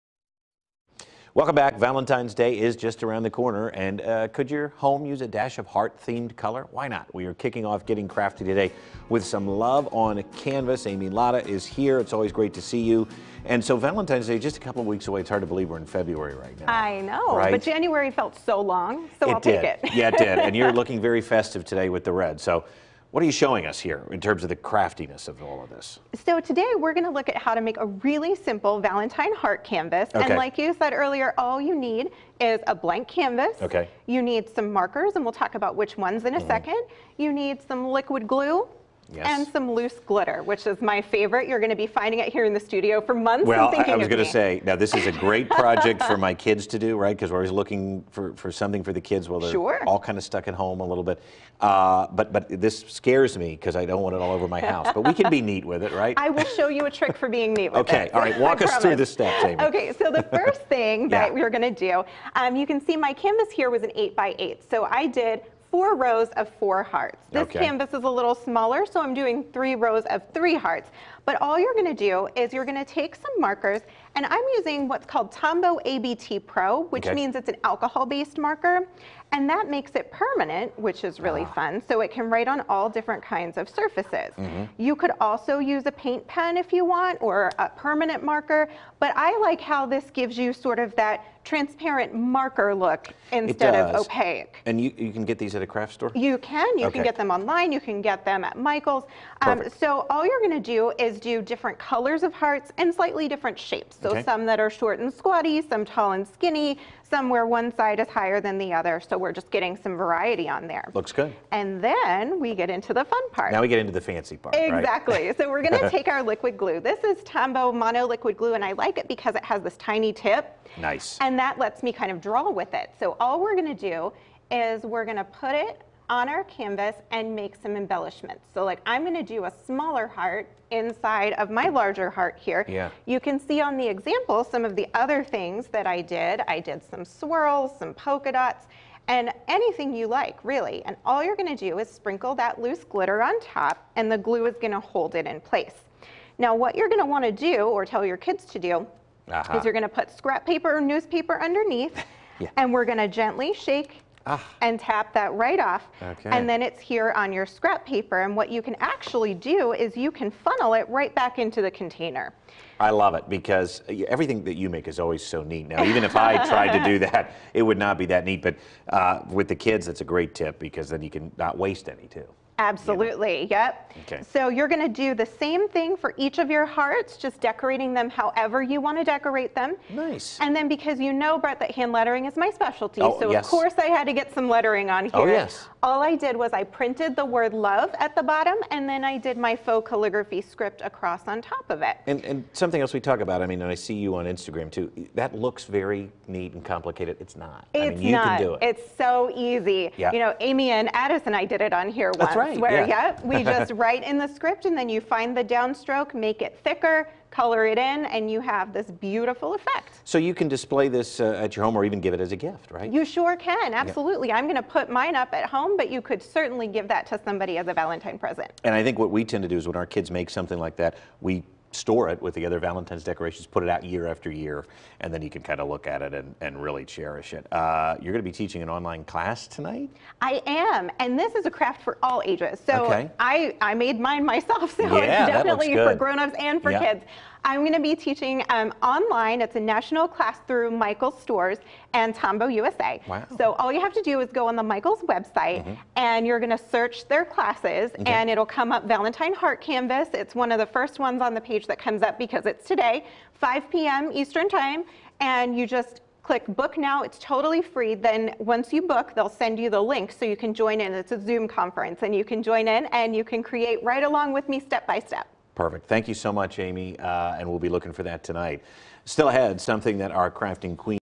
Valentine Heart Canvas
Make this fun and festive Valentine heart canvas using canvas, markers, glue, and glitter!